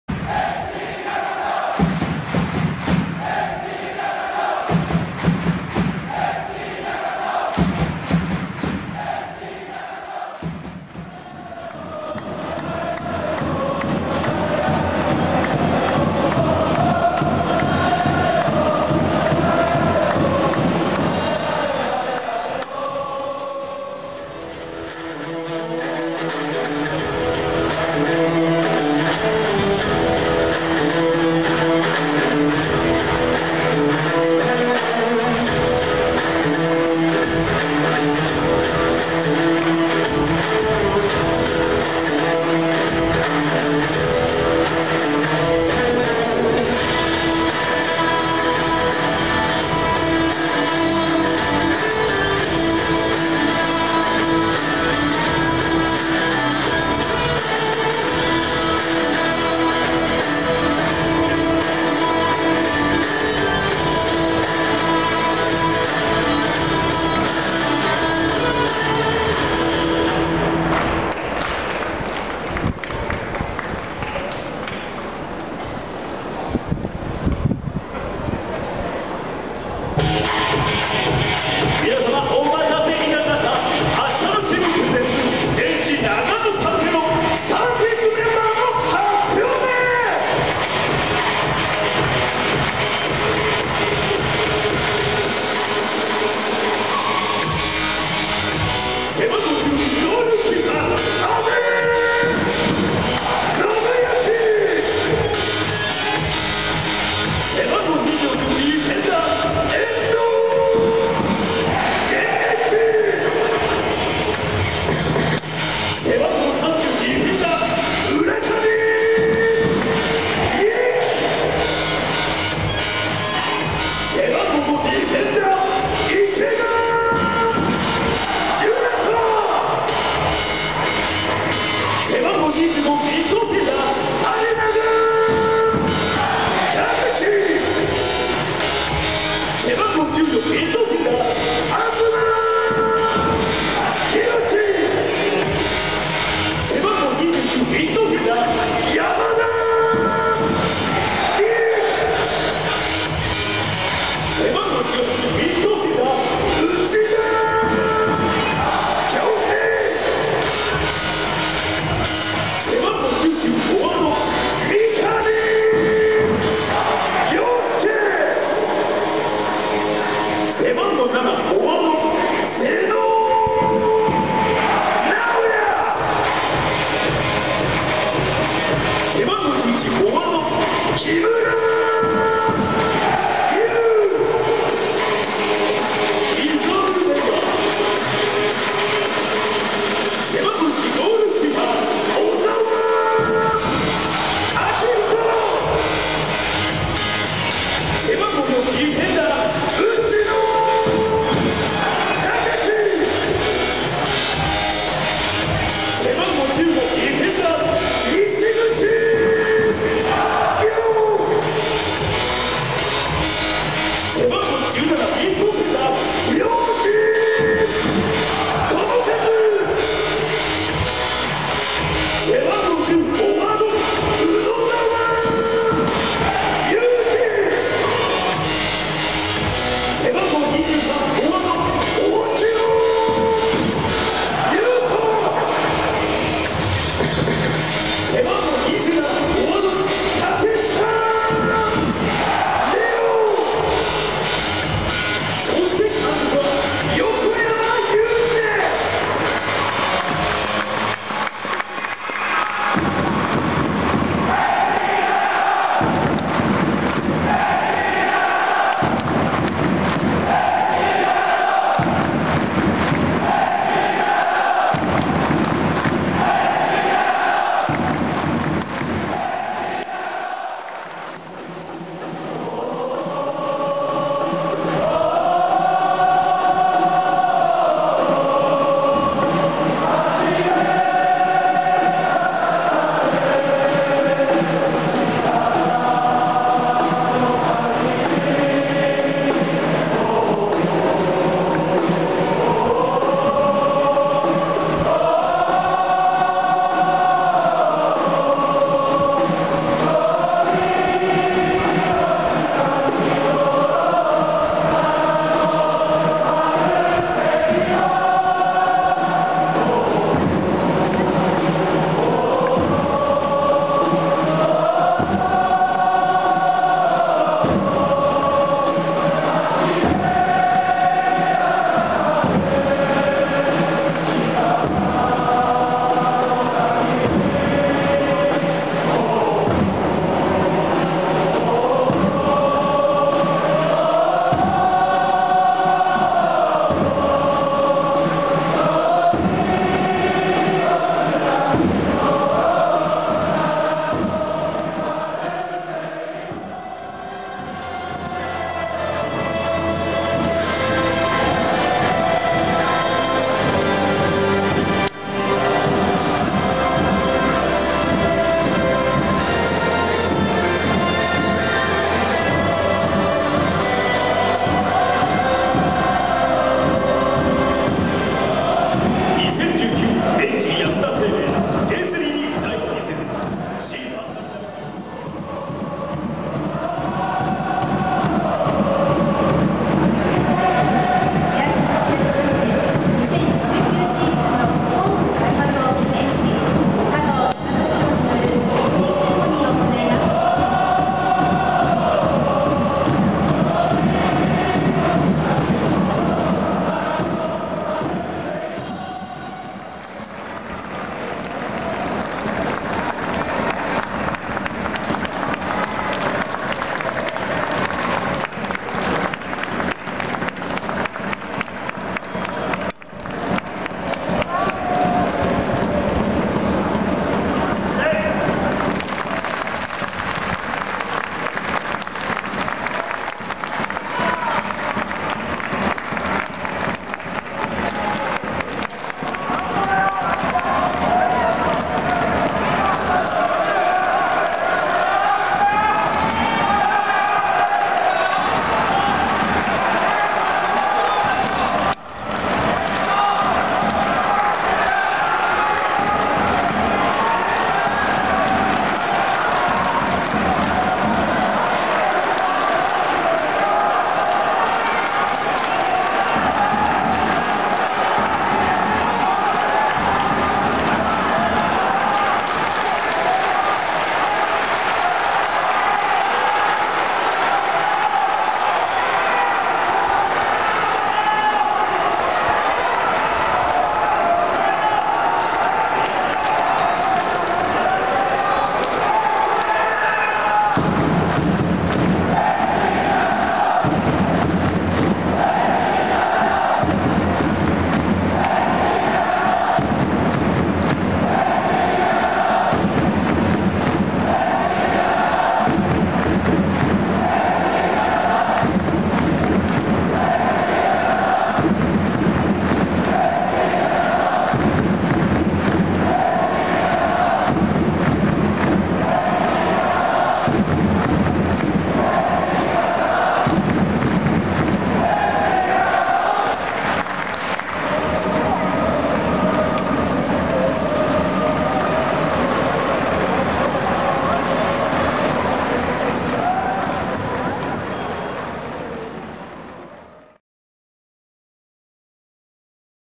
AC長野パルセイロ2019ホーム開幕戦 選手紹介 sky 選手挨拶
2019 明治安田生命 J3 リーグ第 2 節
AC長野パルセイロ vs ヴァンラーレ八戸
長野Uスタジアム